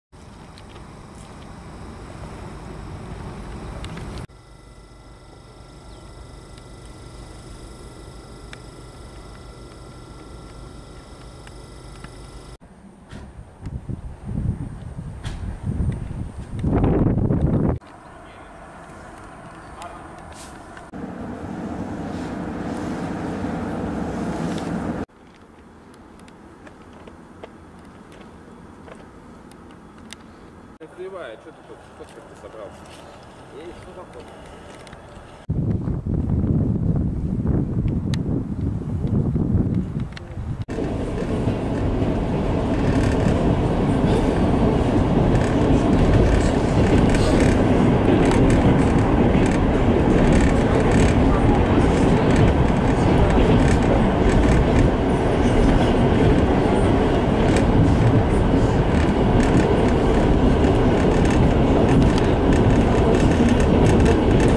Электропоезд "Иволга 3.0" ЭГЭ2ТВ-001 у станции Подольск
Электропоезд Иволга 3.0 стояла неподалёку от станции Подольск, МЦД-2.